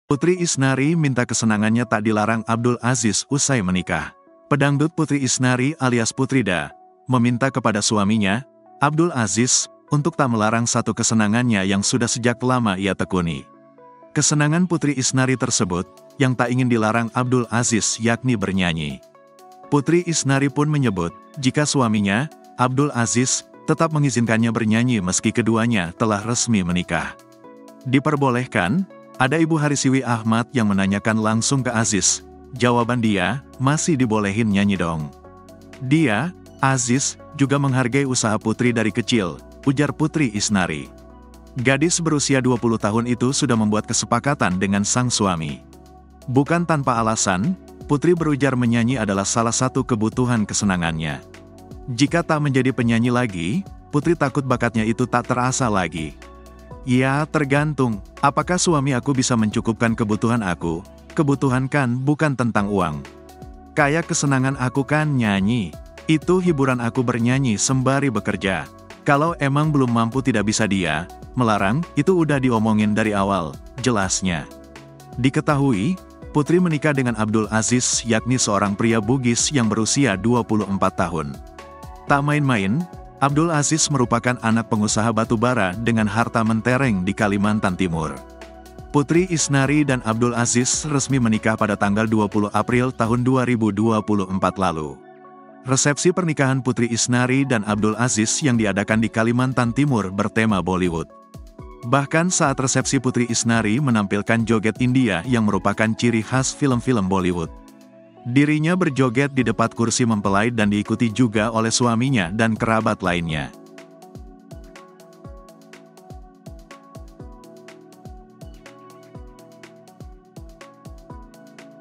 Putri Isnari Minta Kesenangannya Tak Dilarang Abdul Aziz usai Menikah • Berita Artis Tribun Lampung
Pedangdut Putri Isnari alias Putri DA, meminta kepada suaminya, Abdul Azis, untuk tak melarang satu kesenangannya yang sudah sejak lama ia tekuni.

Kesenangan Putri Isnari tersebut, yang tak ingin dilarang Abdul Azis yakni bernyanyi.

Putri Isnari pun menyebut, jika suaminya, Abdul Azis, tetap mengizinkannya bernyanyi meski keduanya telah resmi menikah.